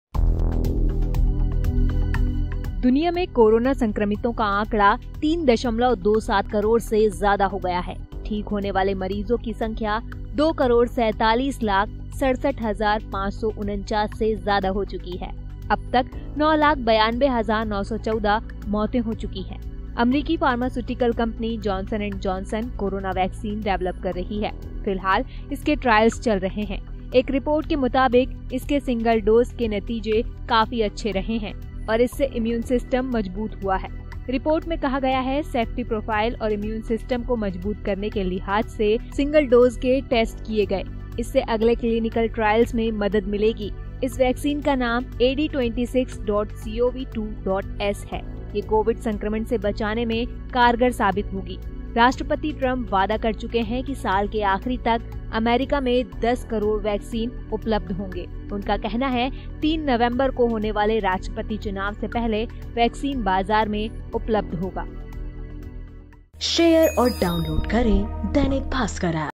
कोरोना वैक्सीन को लेकर अच्छी खबर
कोरोना दुनिया में: अमेरिकी कंपनी जॉनसन एंड जॉनसन की वैक्सीन के ट्रायल्स के अच्छे नतीजे, सिंगल डोज से इम्यून सिस्टम मजबूत हुआ; दुनिया में 3.27 करोड़ केस